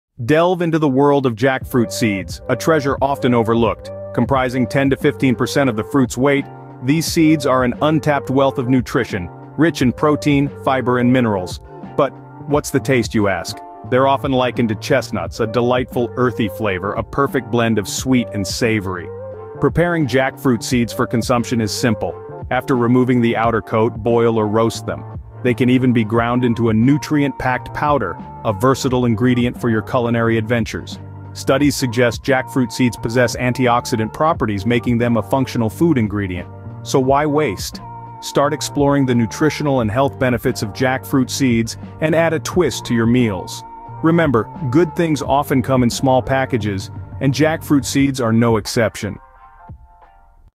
Unveiling the Jackfruit Seed: A Hidden Nutritional Gem
In this exciting video, we delve into the world of jackfruit seeds - the nutritious powerhouse that often goes unnoticed! Join us as we uncover the incredible health benefits and versatile culinary uses of jackfruit seeds that will surely surprise you.